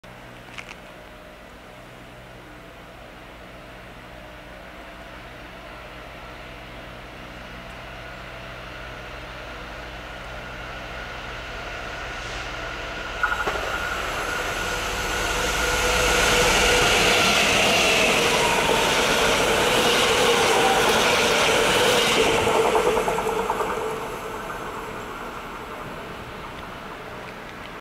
Freightliner 513-02 komt met Arriva Winks 614 en 615 door station Wijhe
Freightliner 513-02 komt met Arriva Winks 614 en 615 door station Wijhe onderweg naar Zwolle en Groningen De Vork.

Vrijdag 18 december 2020 NL